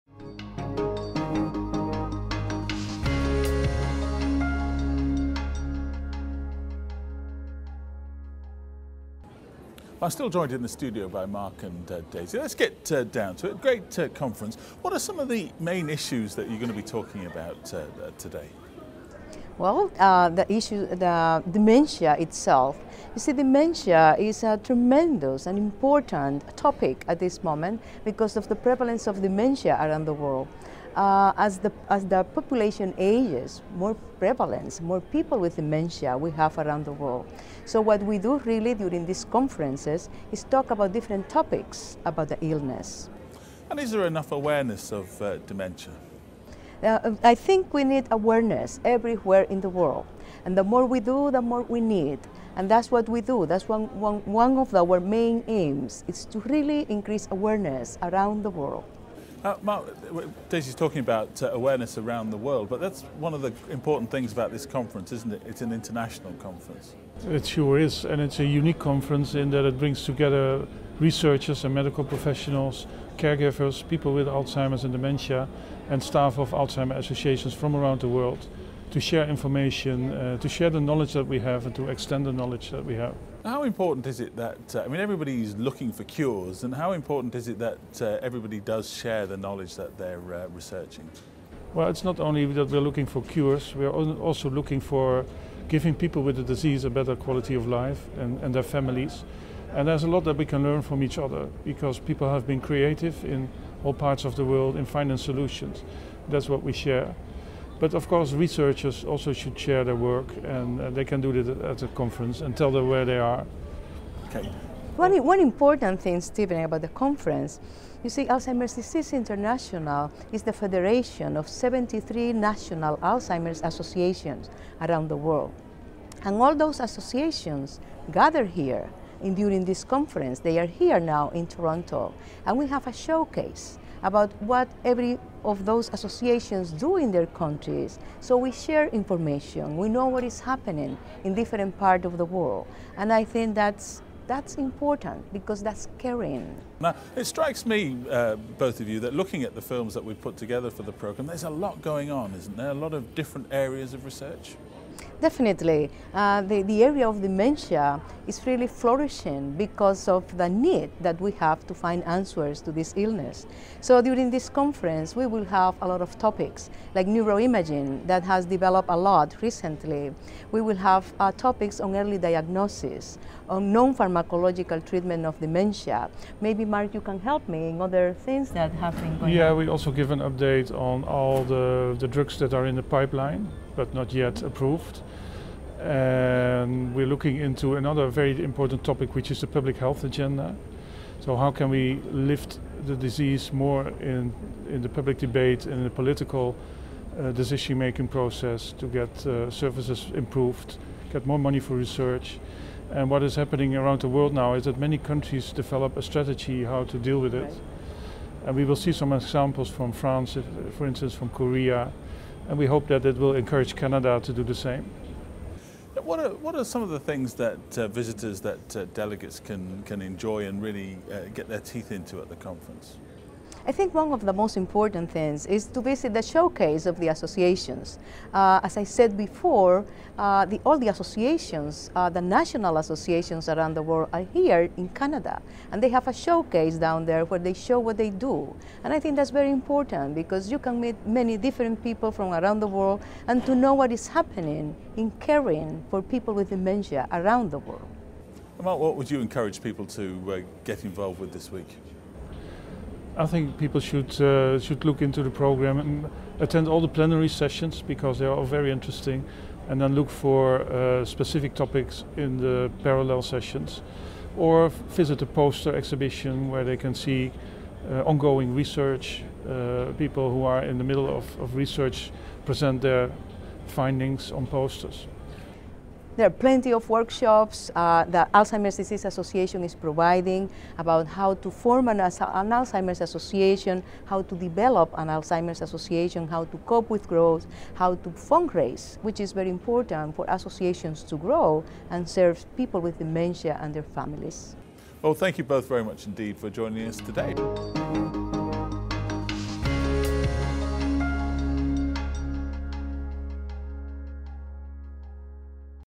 ADI Chair and Executive Director talk to ADI2012 TV
ADI2012 TV catch up with Chair of the ADI, Daisy Acosta along with Executive Director of the ADI, Marc Wortmann about the themes of this year’s Alzheimer's Disease International event and what delegates can look forward to at London’s ExCel centre.